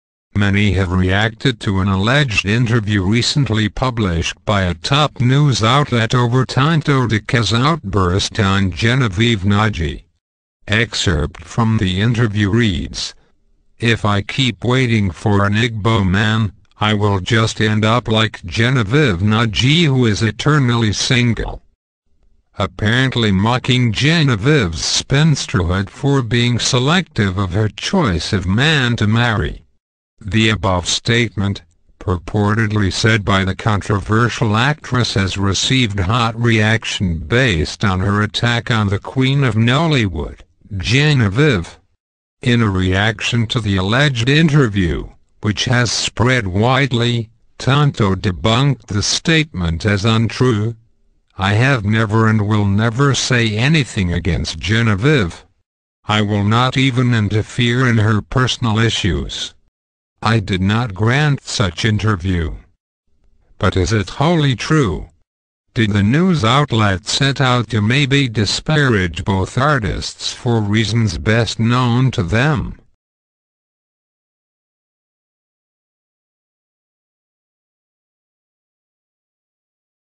Tonto Dike reacts to allegation; \"I don\'t have a problem with Genevieve\"
Many have reacted to an alleged interview recently published by a top news outlet over Tonto Dikeh\'s outburst on Genevieve Nnaji. Excerpt from the interview reads; \"If I keep waiting for an Igbo man, I will just end up like Genevieve Nnaji who is eternally single.\" Apparently mocking Genevieve\'s spinsterhood for being selective of her choice of man to marry. The above statement, purportedly said by the controversial actress has received hot reaction based on her attack on the queen of Nollywood, Genevieve. In a reaction to the alleged interview, which has spread widely, Tonto debunked the statement as untrue; \"I have never and will never say anything against Genevieve. I will not even interfere in her personal issues. I did not grant such interview.\" But is it wholly true? Did the news outlet set out to maybe disparage both artistes for reasons best known to them?